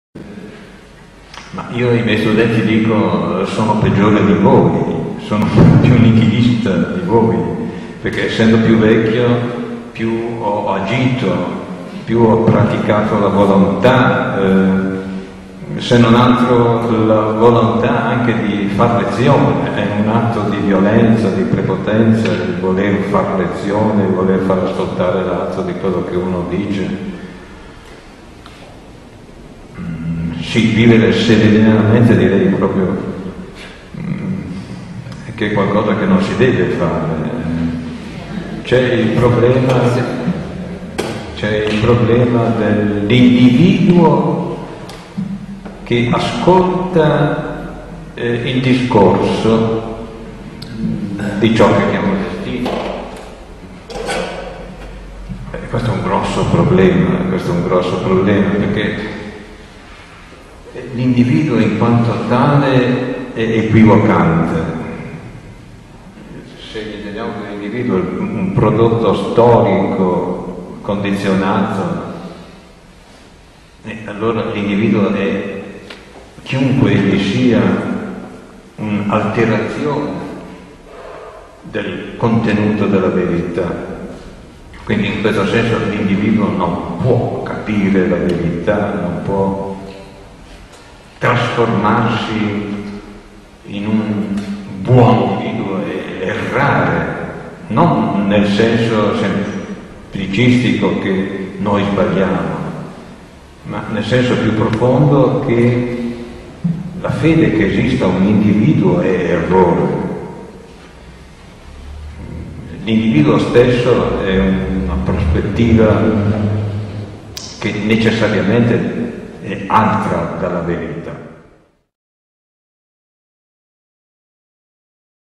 Porta Celeste, Emanuele Severino, ' Parla in occasione dei suoi ottant'anni', 6 maggio 2009
Riprese e montaggio di Giovanna Pastorelli. Tutti i diritti a GruppoA